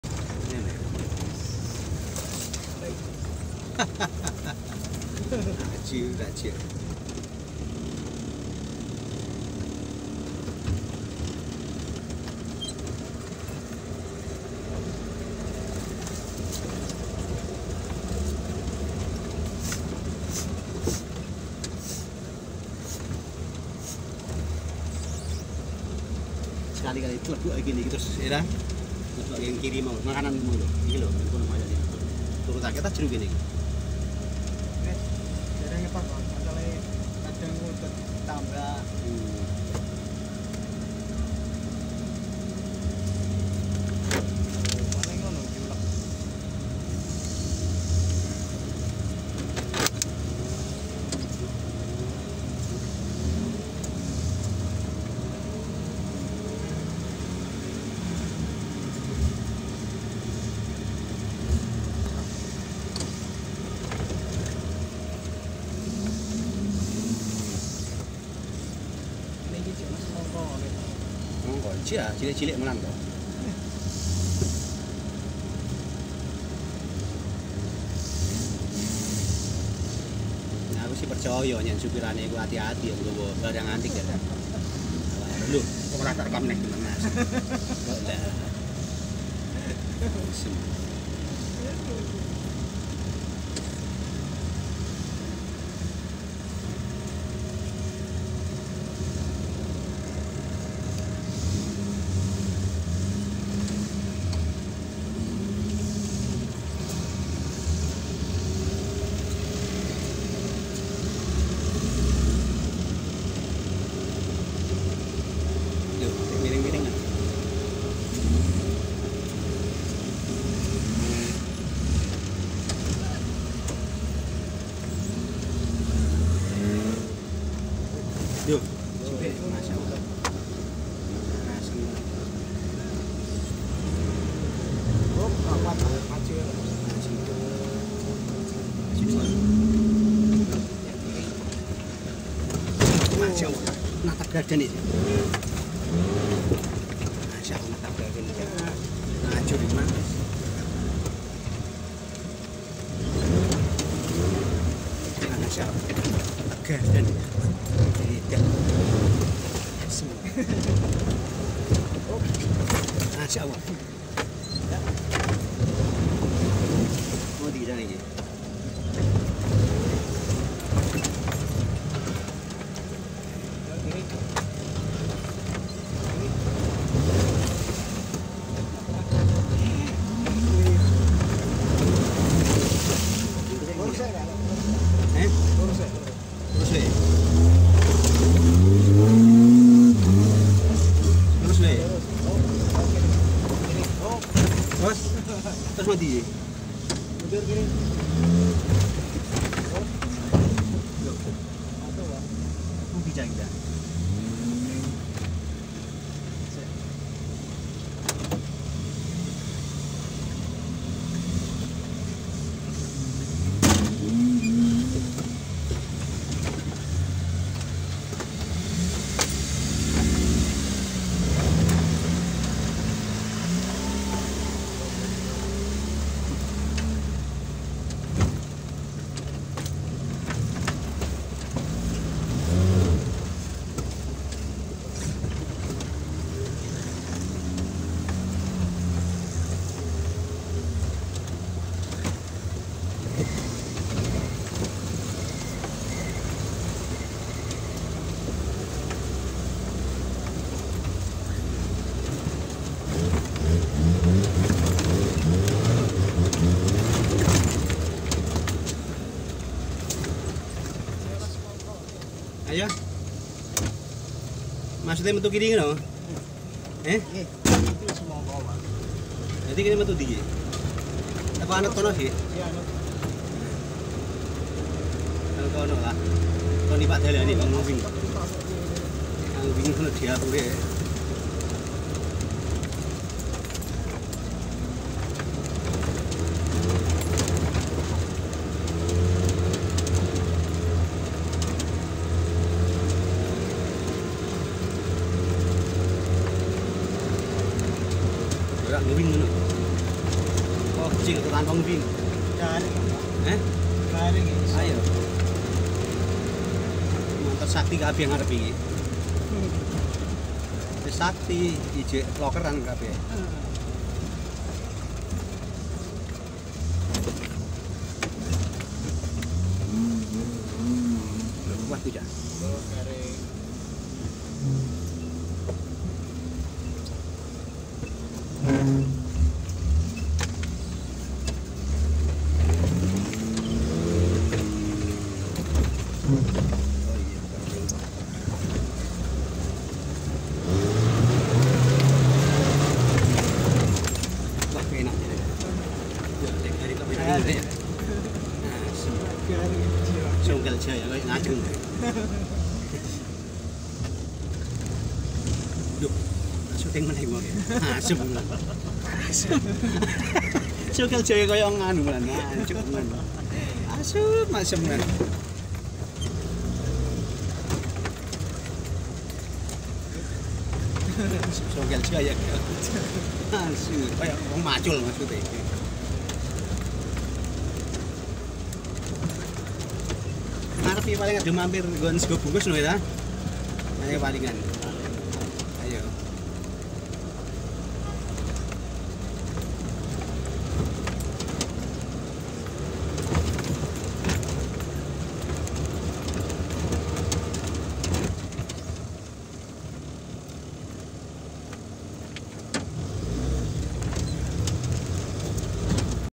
Nyeberang sungai Batu songkel Jaya | offroad extreme
off-road Extreme malam hari melalui track sungai dan tanjakan di desa suminding , perjalan offroad dimulai jam 10 malam sampai finish jam 2.30 di desa Bangsri